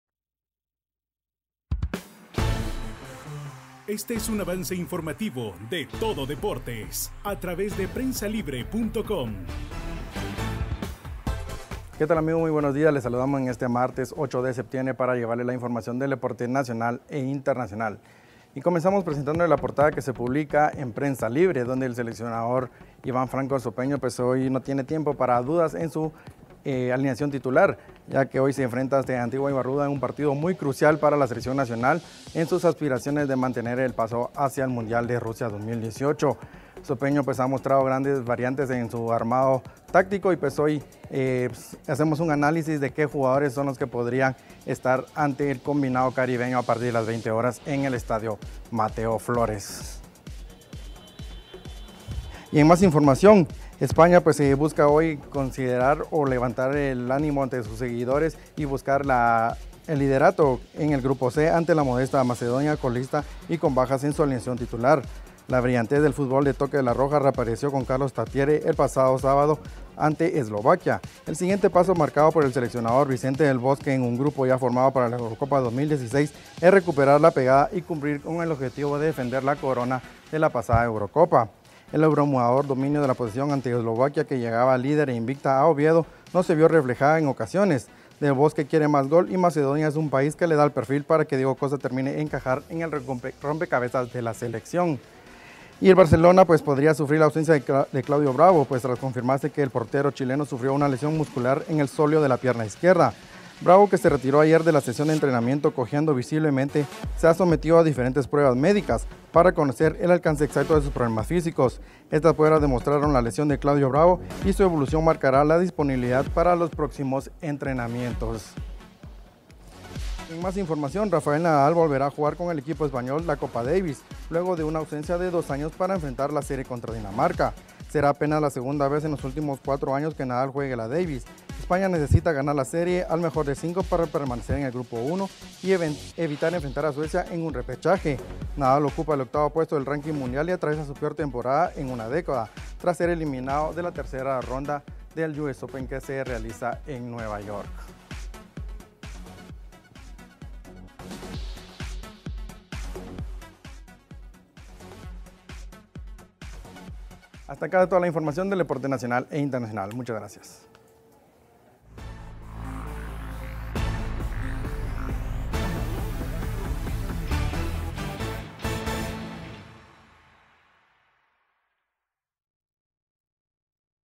Selección obligada a ganar
Selecciona Nacional obligada a ganar con un equipo ofensivo | La selección española se prepara para la defensa de la corona en la Eurocopa 2016 | El Barsa podría tener la baja de Claudio Bravo en el encuentro frente al Atlético de Madrid | Rafael Nadal volverá a jugar en el equipo español de la Copa Davis